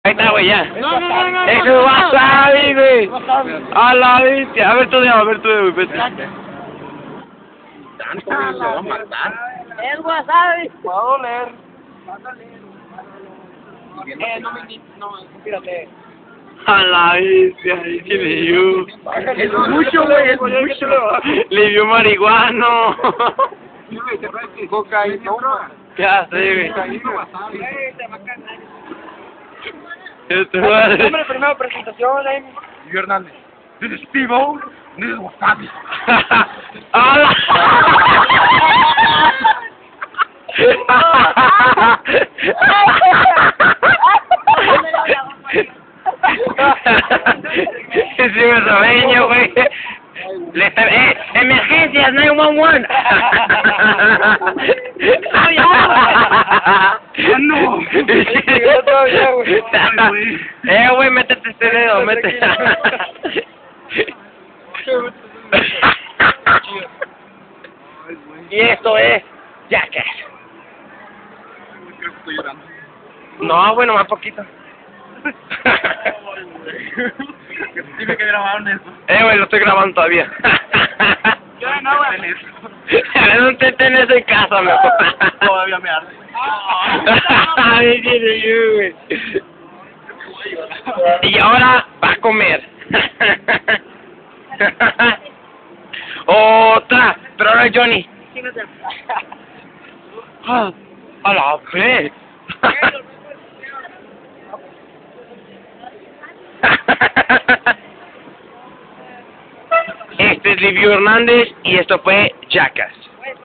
liviu and wasabe
liviu making jackass at the mall in Nogales he is going to inhale wasabe